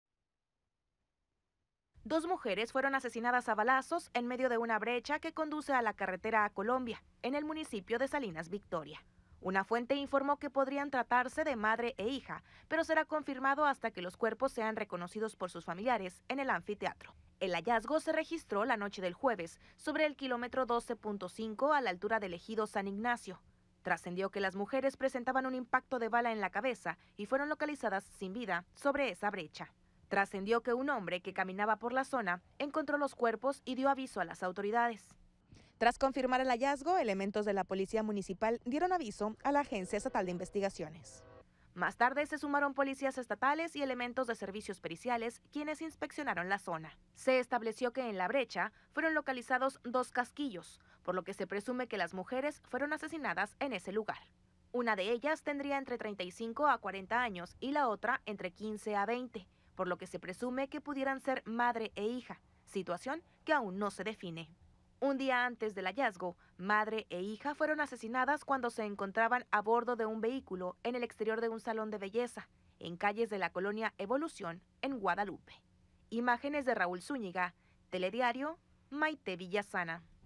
Encuentran cuerpos de dos mujeres en Salinas Victoria
Dos mujeres fueron asesinadas a balazos en medio de una brecha que conduce a la Carretera a Colombia, en el municipio de Salinas Victoria.

Una fuente informó que podrían tratarse de madre e hija, pero será hasta que los cuerpos sean reconocidos por sus familiares en el anfiteatro.

El hallazgo se registró la noche del jueves, sobre el kilómetro 12.5, a la altura del Ejido San Ignacio.

Programación
Lunes a viernes:
Telediario Matutino de 05:00- 10:00
Telediario Mediodía de 12:00-14:30
Telediario Nocturno de 19:00- 21:00